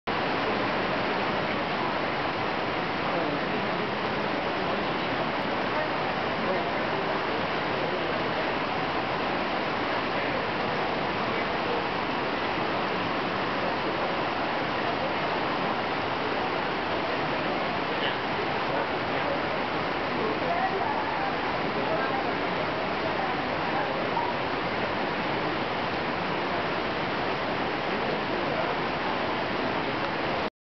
29/07/2007 Nikko Train Station (Japan)
颱風來臨時在日本栃木県的日光駛.

Typhoon approaching while waiting at Nikko (日光) train station in Tochigi Prefecture (栃木県), Japan (日本).